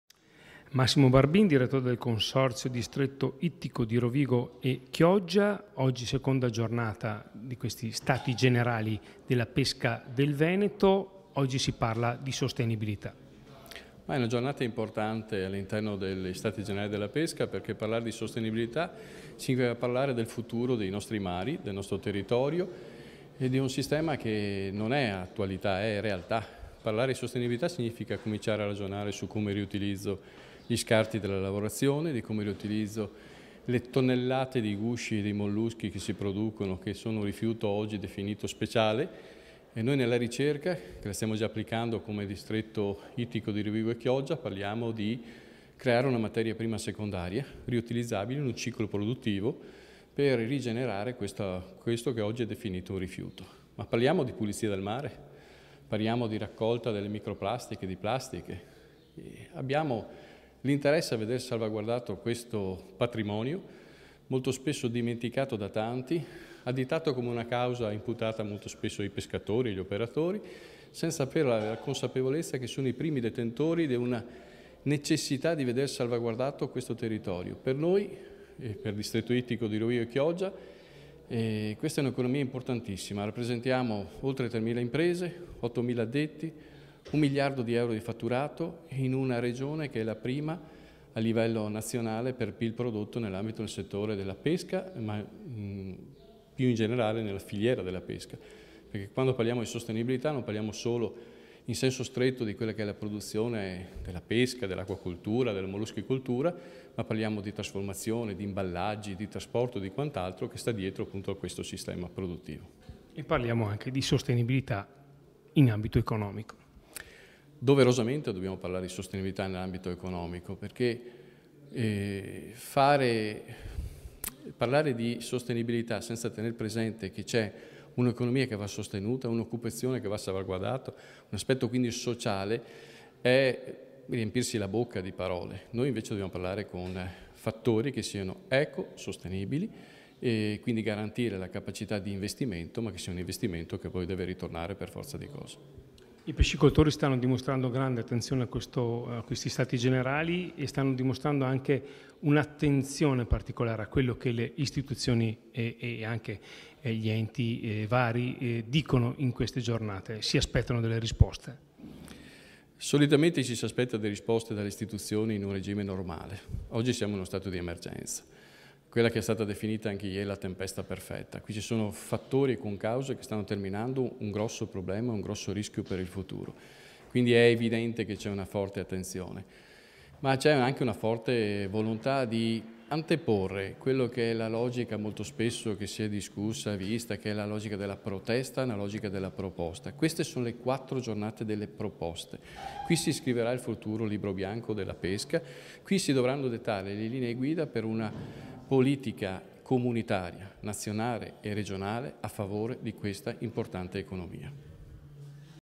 Massimo Barbin, Consorzio Distretto Ittico di Rovigo-Chioggia |Stati Generali della Pesca del Veneto
Massimo Barbin, Consorzio Distretto Ittico di Rovigo-Chioggia | Stati Generali della Pesca del Veneto 2022

mercoledì 6 luglio | PORTO TOLLE (RO) | Mercato Ittico di Pila

LA CRESCITA BLU E LA RICERCA DI UN MODELLO DI ELEVATA SOSTENIBILITÀ AMBIENTALE, ECONOMICA E SOCIALE

Il secondo appuntamento degli Stati Generali della Pesca del Veneto si è tenuto al Mercato Ittico di Pila nella località di Porto Tolle ed era dedicato a una delle tematiche più cruciali di questo secolo: la sostenibilità vista secondo le tre declinazioni ambientale, economica e sociale. Dall’esperienza maturata dagli operatori in particolare nella realizzazione dei progetti FEAMP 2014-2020 si potranno elaborare utili indicazioni per un modello di sviluppo fondato su un’economia blu sostenibile.